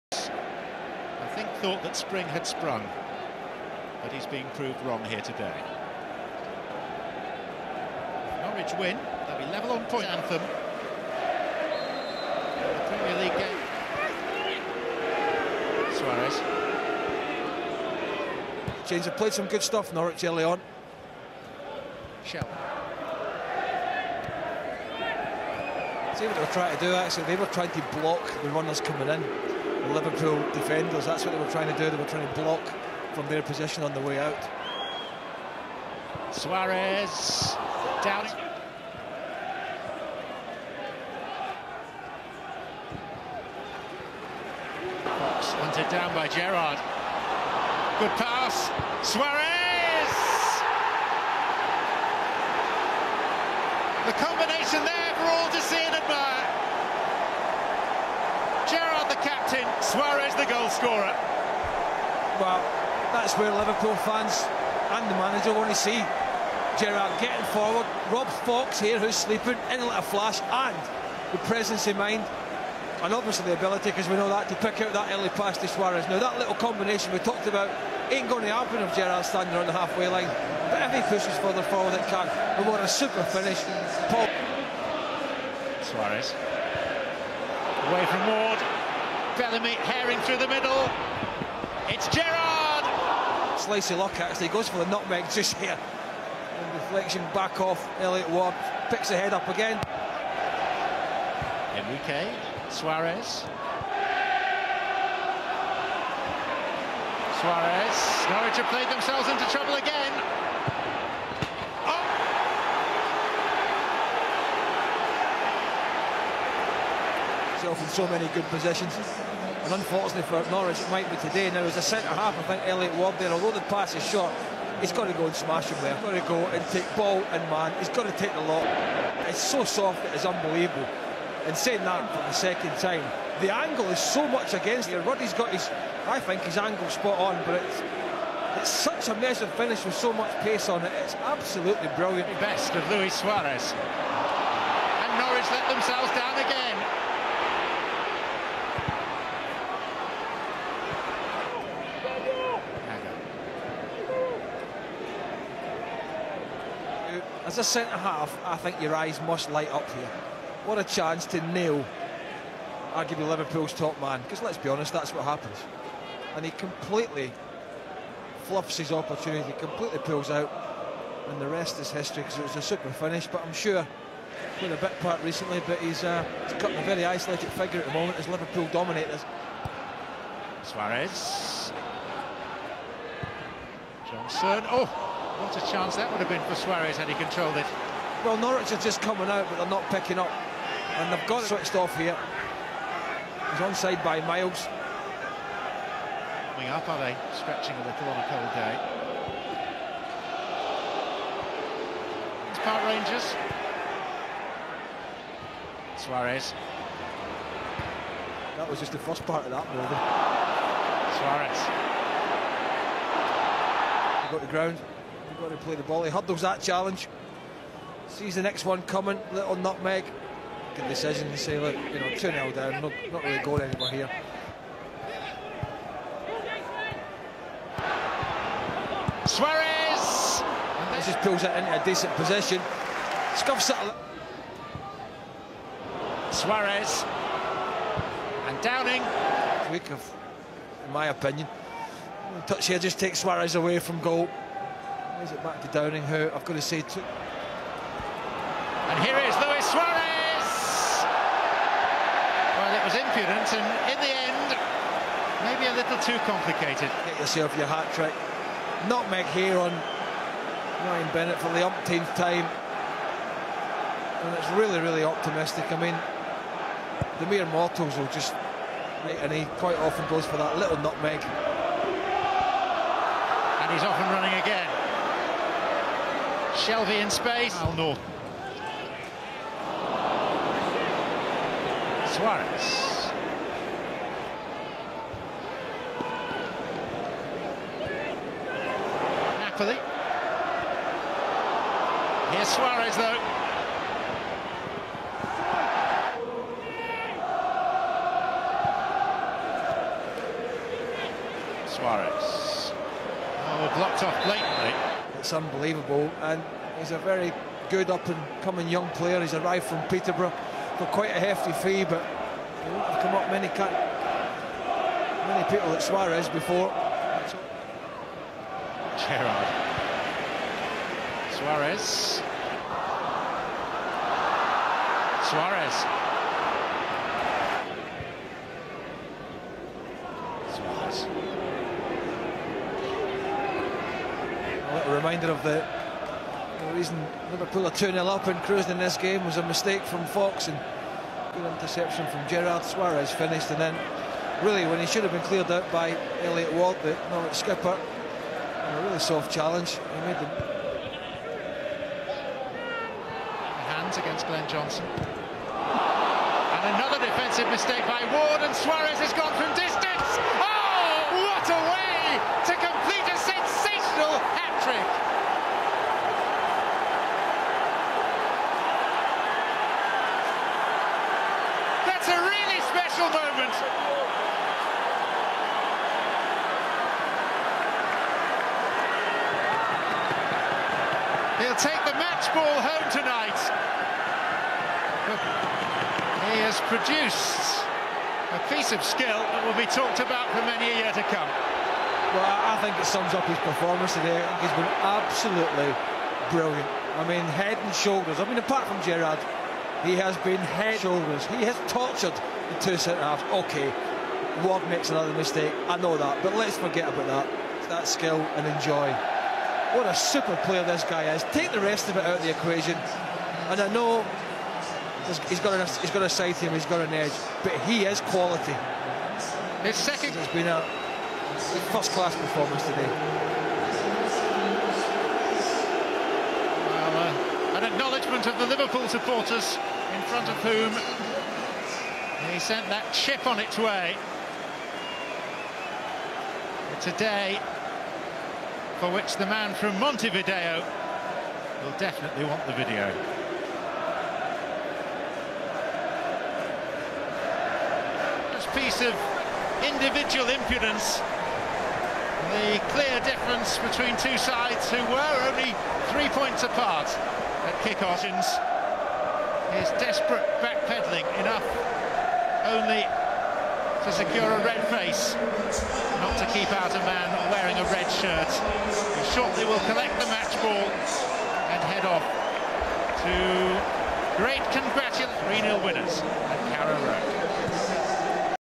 Luis Suarez vs Norwich (A) 2011/2012 | Hat-Trick (English Commentary) HD
Norwich v Liverpool 0-3 2012 at Carrow Road. Luis Suarez produces one of his best displays in a Liverpool shirt by scoring his first Liverpool hattrick, all being sublime finishes. The last being from the half way line.